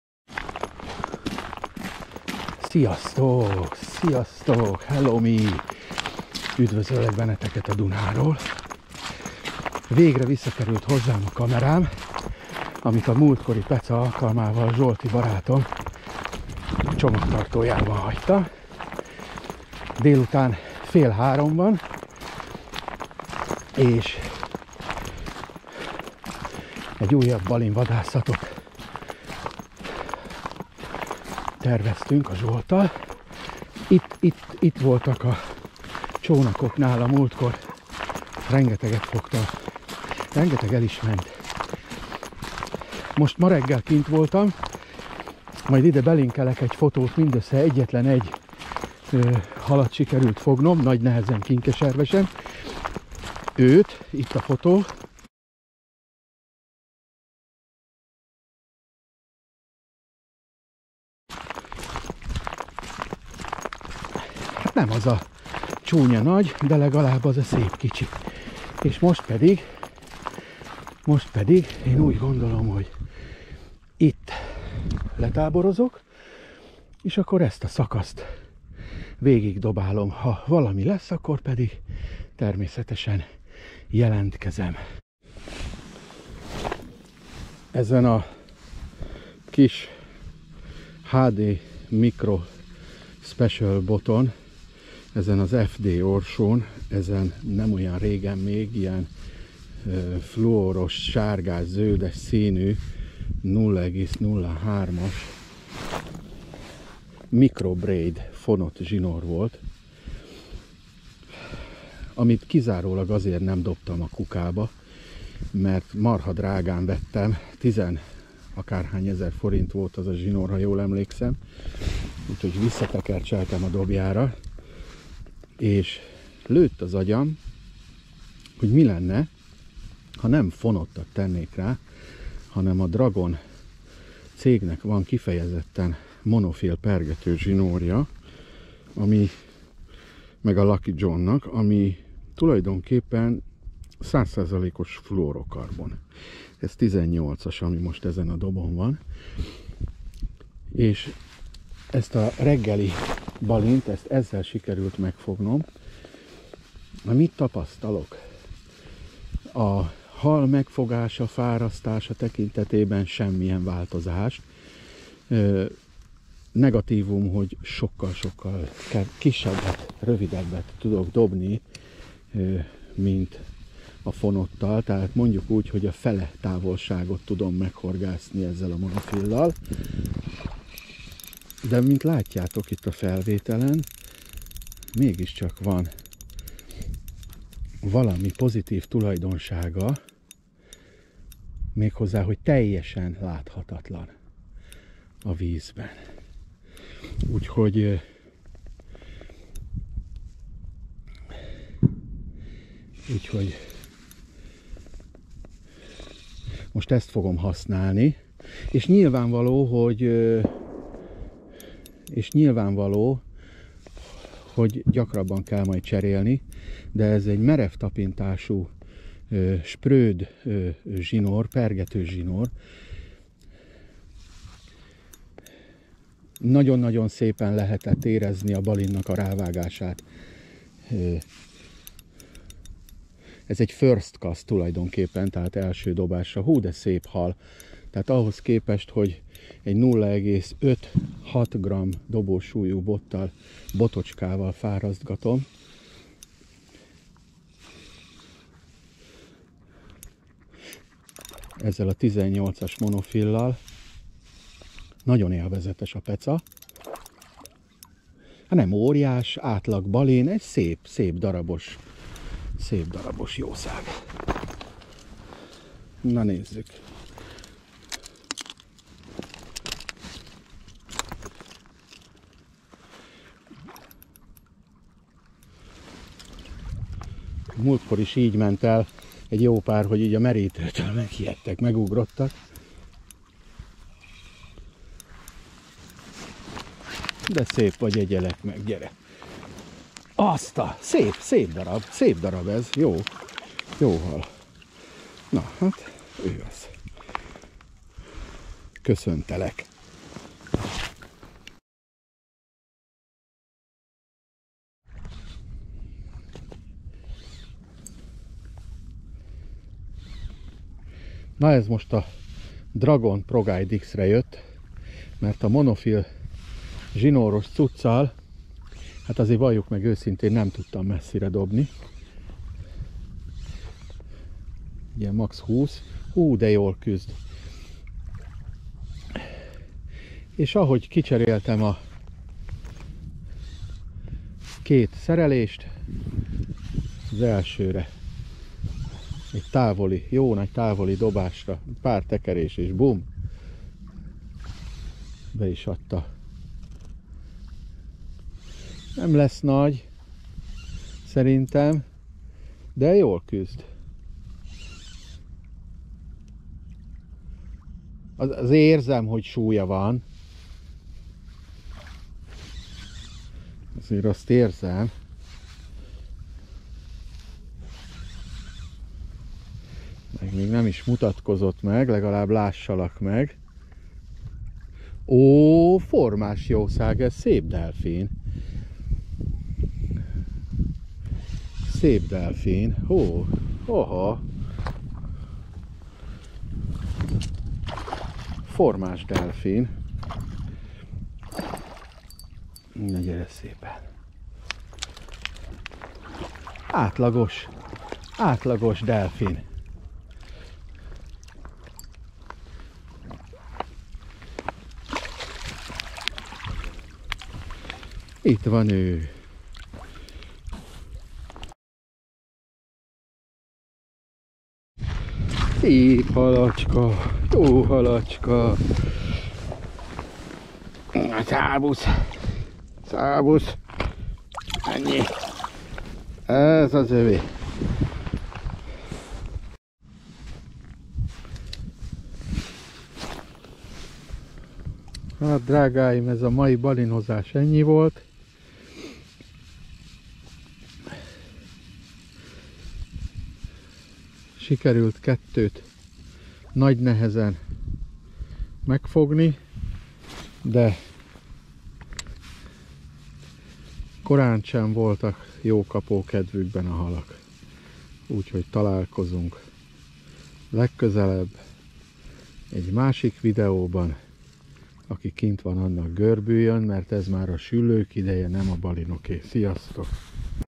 Nem minden nap egyforma Spinning for Asp Fishing on Duna river
A balin természetesen nem az első dobásra jött, hanem kb az ötödikre, de nincs jelentősége. :)